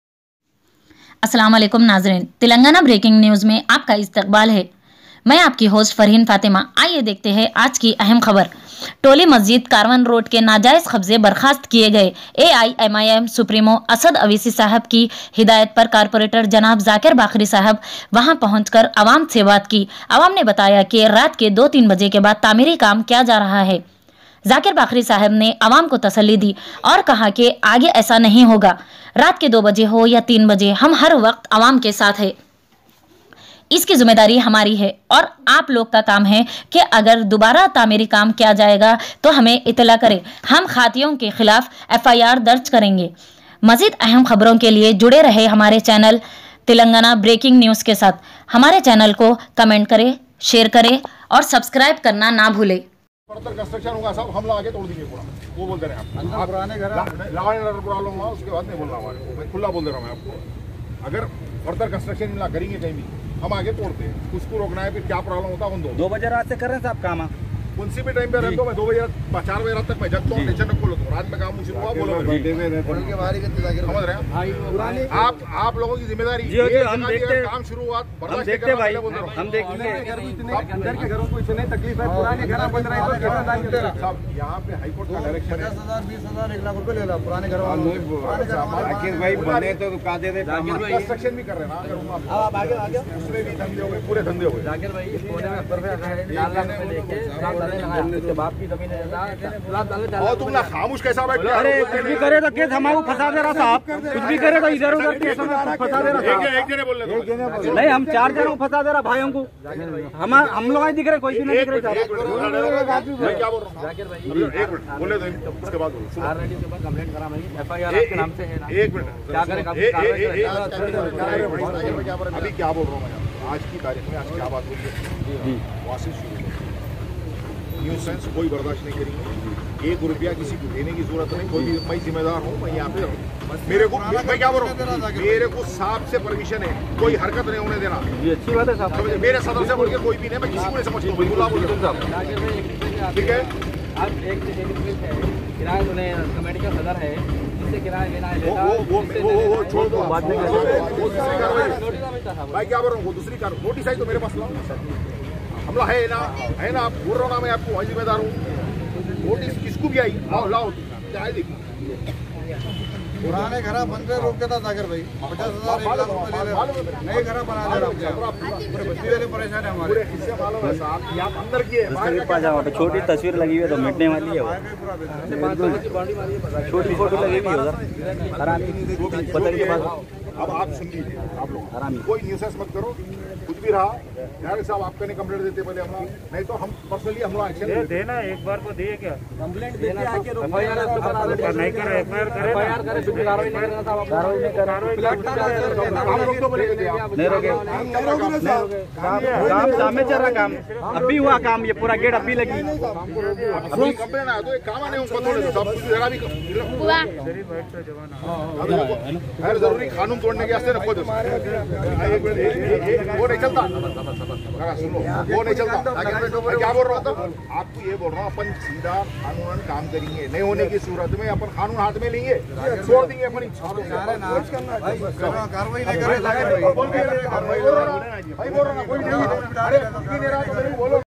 TOLIMASJID KE najayas khabze bharkhas kiye gaye zakir Baqueri AIMIM corporater asad sab ki hidayat p
TOLIMASJID KE najayas khabze bharkhast kiye gaye janab zakir Baqueri sahab AIMIM corporater asad sab ki hidayat pe